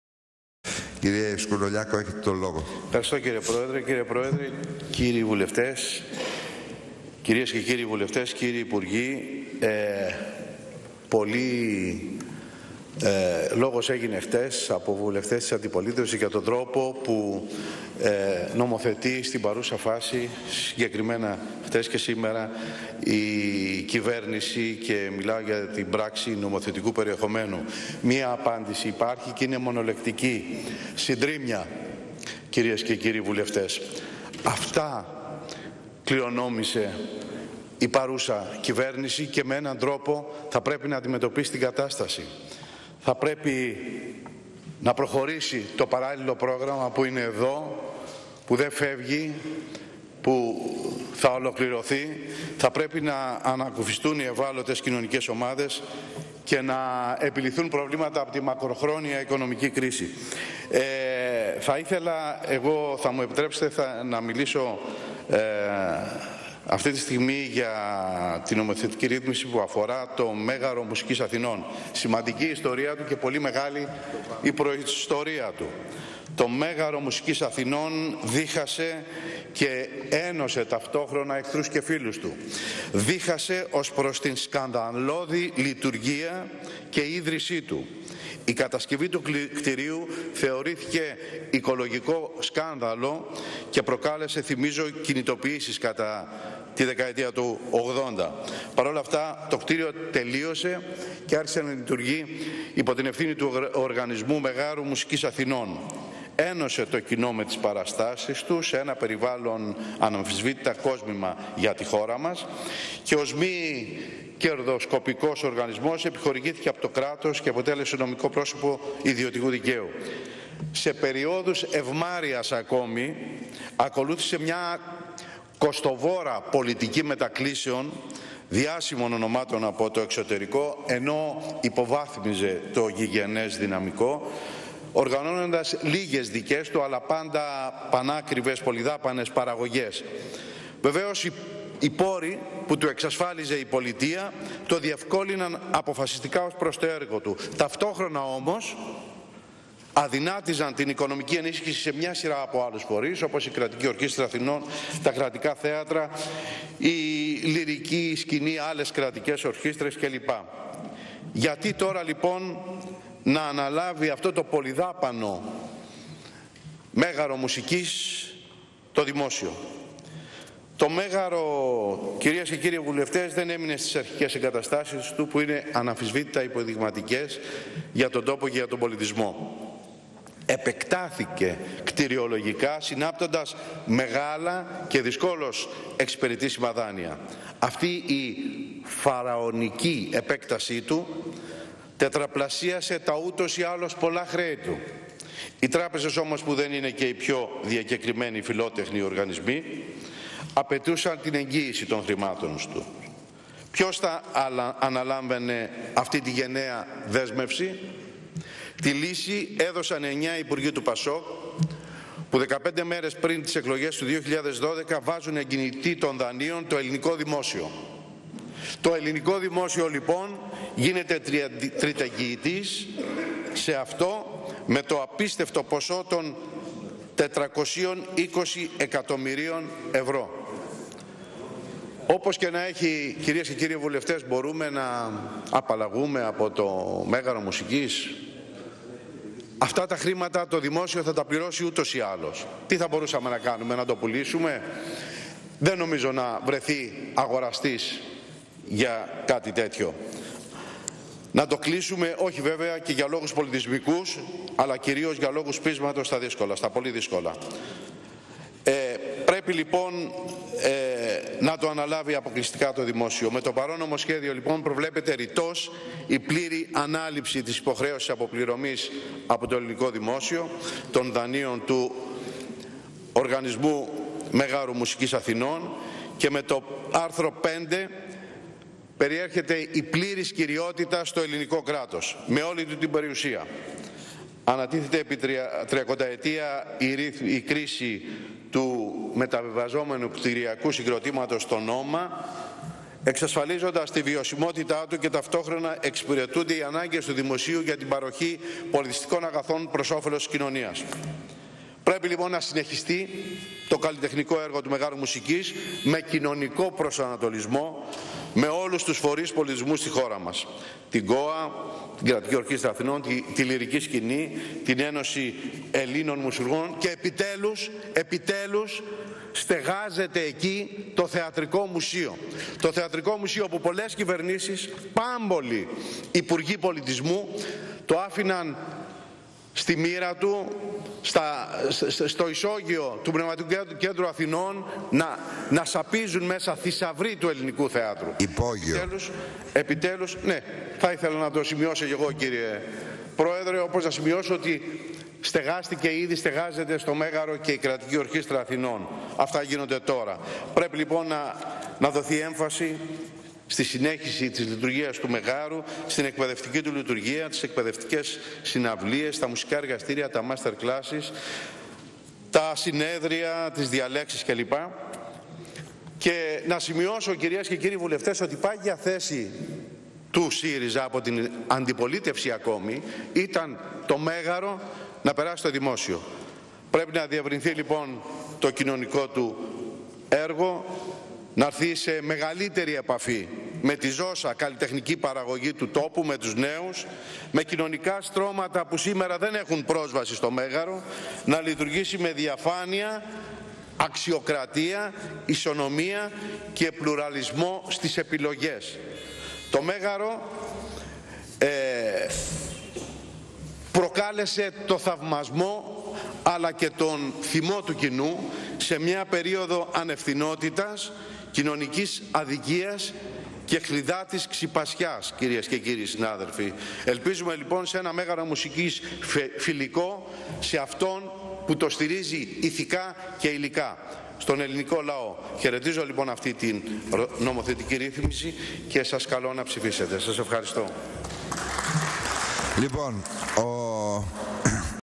Ομιλία του Πάνου Σκουρολιάκου για την κατατεθείσα Τροπολογία σχετικά με το Μέγαρο Μουσικής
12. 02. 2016  Ομιλία του Πάνου Σκουρολιάκου για την κατατεθείσα Τροπολογία σχετικά με το Μέγαρο Μουσικής